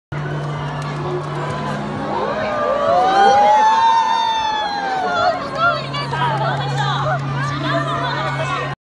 ミミズクが目の前を！ 神戸どうぶつ王国バードショー
ミミズクが観客席の目の前を通過します。